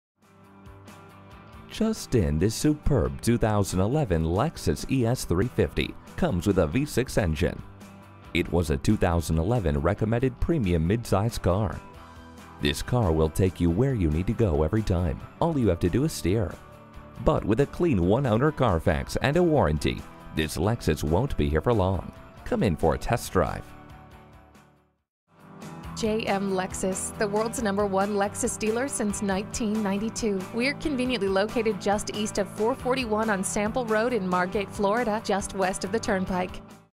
2011 Lexus ES 350 Margate FL Ft-Lauderdale, FL 42199A SOLD
Year: 2011
Make: Lexus
Model: ES 350
Trim: Base
Engine: 3.5L V6
Transmission: Automatic 6-Speed
Color: Starfire Pearl
Interior: Tan
Mileage: 38402
Stock #: 42199A
!! 1 OWNER / CLEAN CARFAX !! LEXUS CERTIFIED! 3 YR/100K WARRANTY! BUY WITH CONFIDENCE! LEATHER INTERIOR!! POWER MOONROOF!! PARKING SENSORS!! BLUETOOTH!! How exclusive is this! Just in this superb 2011 Lexus ES comes with a 3.5L V6 DOHC Dual VVT-i 24V engine and FWD. Consumer Guide named the ES a 2011 Recommended Premium Midsize Car! This car will take you where you need to go every time...all you have to do is steer!

Address:
5350 W Sample Rd